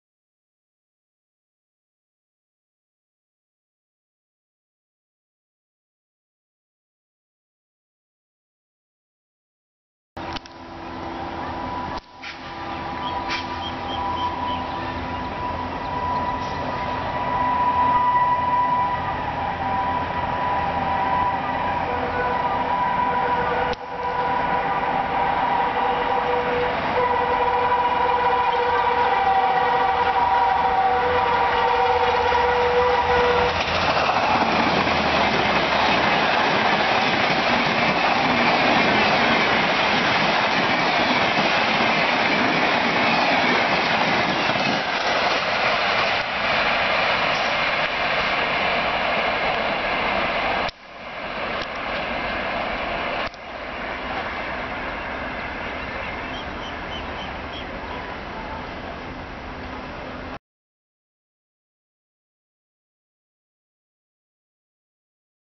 The Electrifying Bhopal Shatabdi Ravages WAP-7 Chattisgarh @ 150+ with Extreme Whining by the Beast
DATE : 18/11/2012
LOCATION : Hodal , along high speed NDLS - MTJ corridor , North Central Railways , Haryana

Finally finally & finally got the rare Super Aggressive 'Jhalak' of the 'Shaan & Jaan' of Indian Railways .
First meet can be so Thrilling , Unimaginable !
Just after 3 mins of loop lining of AJNI WAP-7 hauled ASR - BSP Chattisgarh Express, 'Power Sprinter' of IR GZB WAP-5 #30034 greets me as if he had no mood of slowing down & completely ruins apart 1 hr late running Chattisgarh Exp & Hodal at full throttle , hauling India's Fastest train 12002 New Delhi - Bhopal Shatabdi Express
All your senses get disabled after witnessing such a speed show in these foggy chilly winters
As expected , The 'Baadshah' shuts up the mouth of all those who disrespect it by showing off its full Fierceness & rage

Enjoy my Maiden upload of The Undisputed King of Indian railways with some terrific whining & track sounds